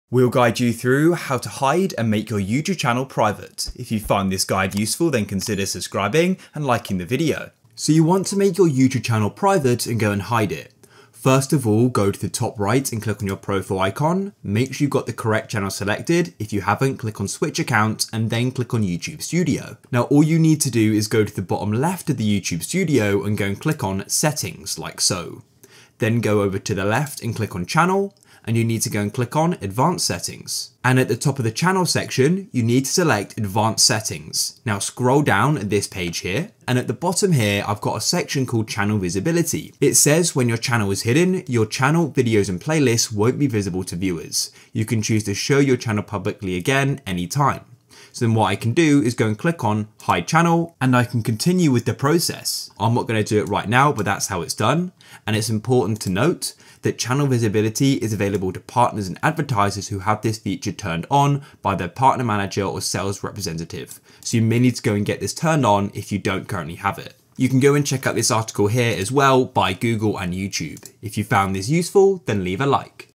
How To Make Your YouTube Channel Private (Hide) - Full Guide
Learn how to make your youtube channel private in this video.

GuideRealm